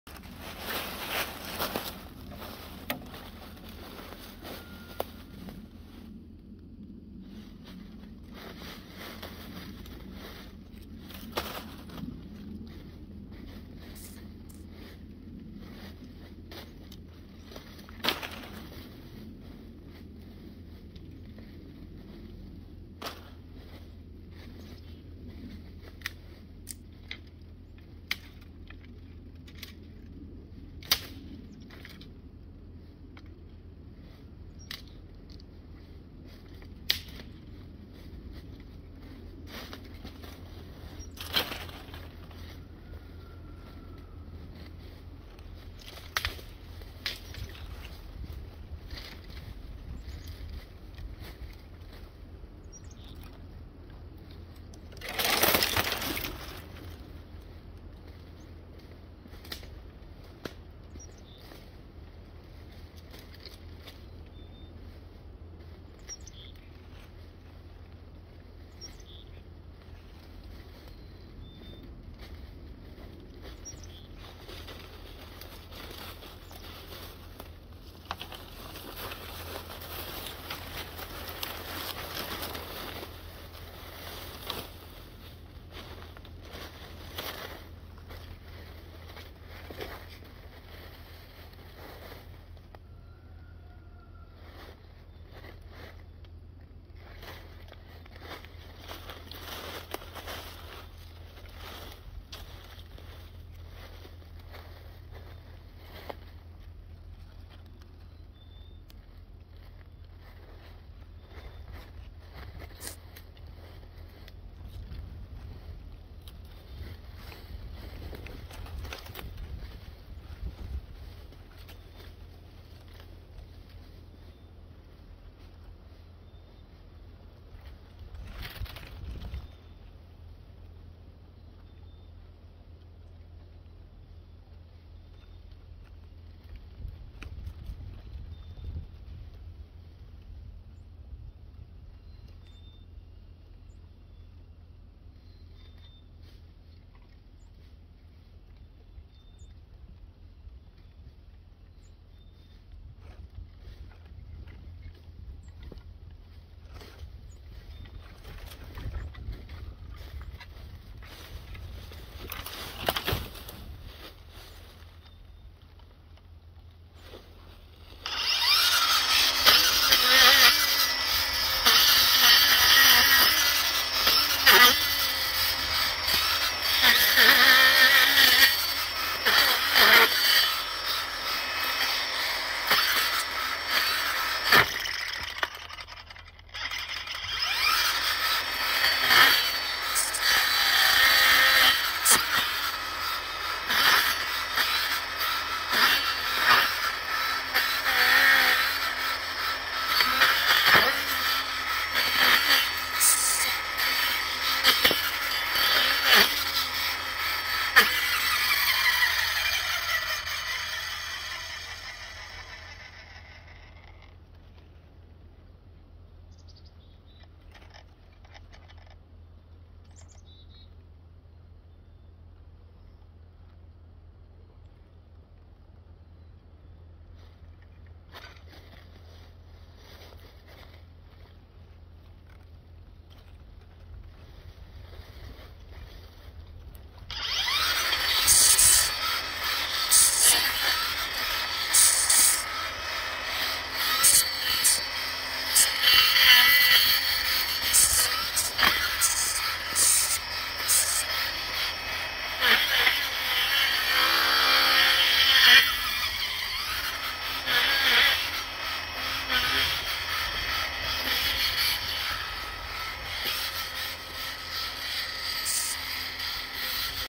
Clearing some woods ft. Dewalt Trimmer
title pretty much said it!

getting a spot cleared for some storage, lots of woods to unwood.